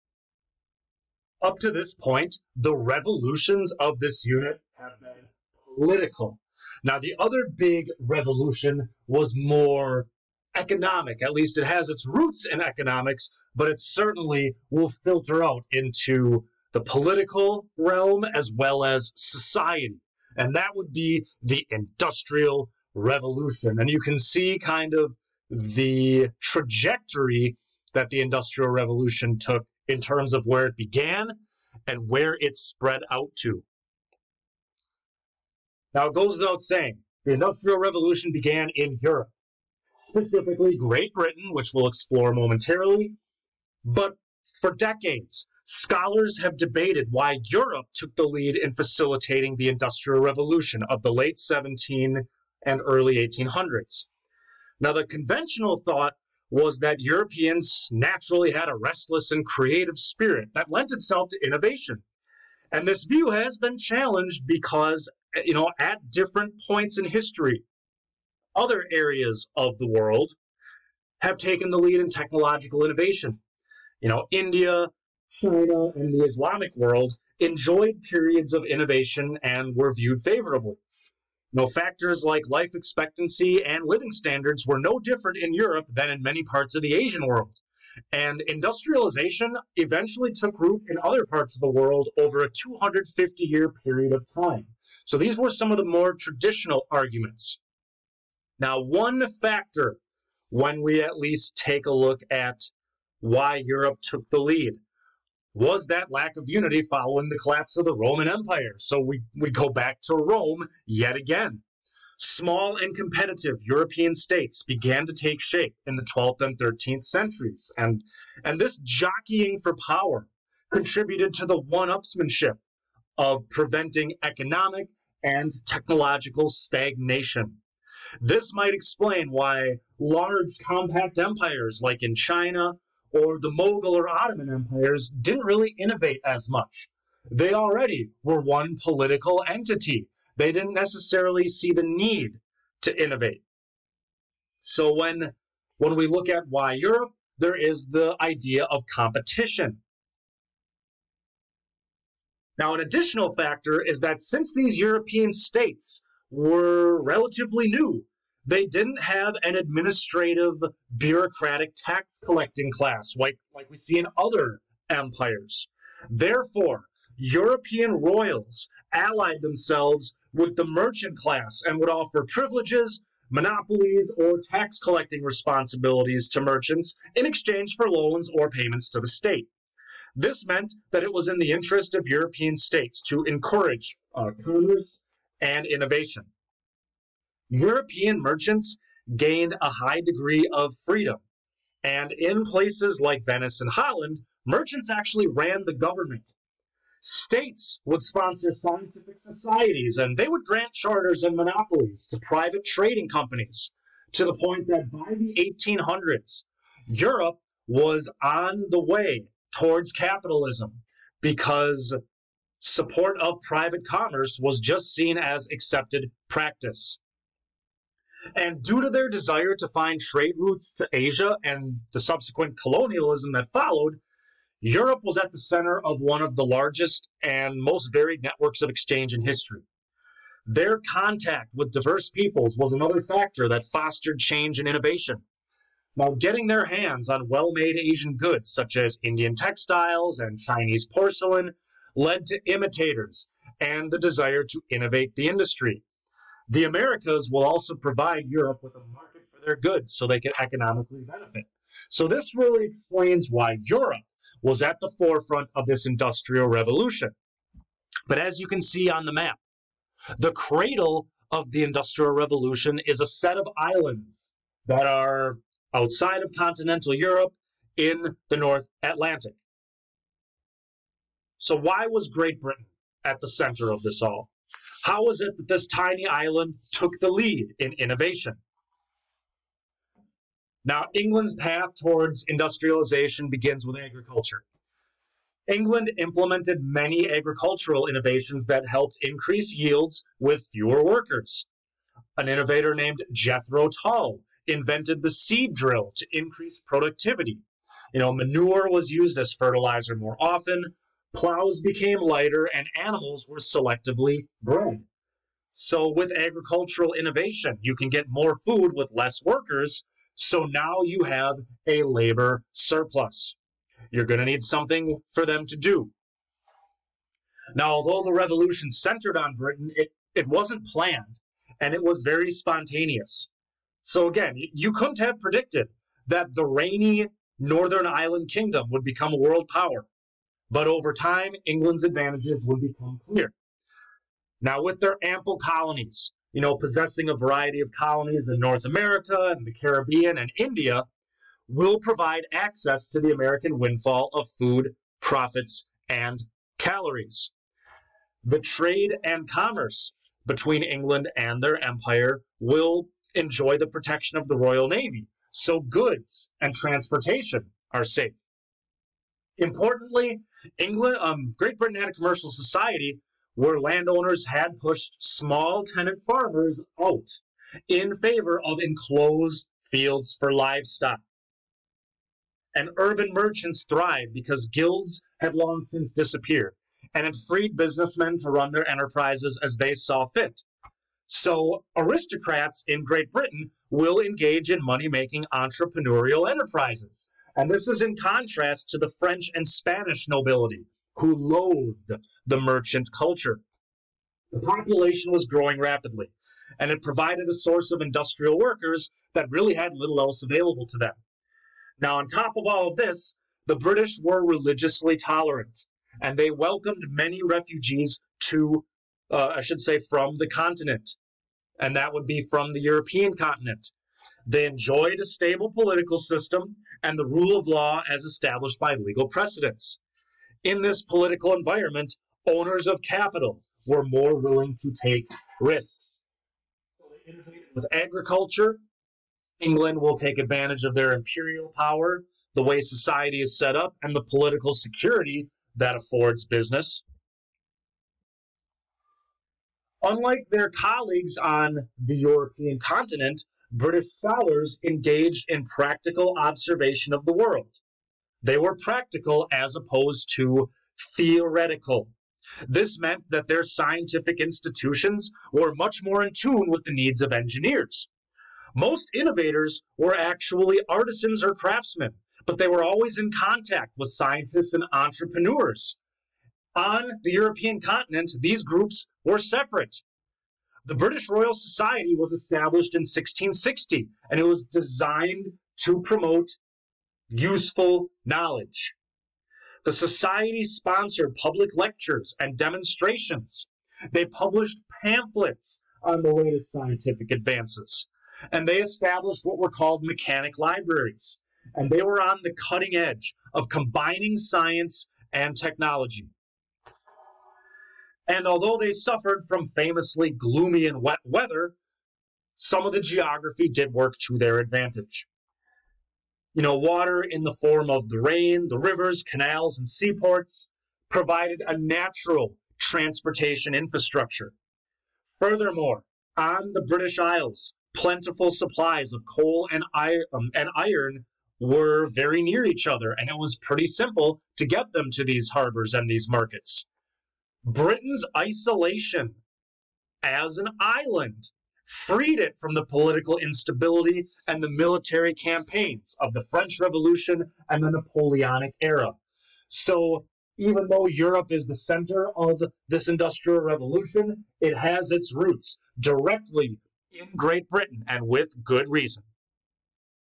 Revolution #6: Industrial Origins: Great Britain's Triumph
Covers the factors that allowed Europe, Great Britain taking the lead, to become the forerunner of the new "Industrial Revolution."

Topic 5.3: Industrial Revolution Begins
Learning Objective: 5:D
Thematic Focus: Humans and the Environment [ENV]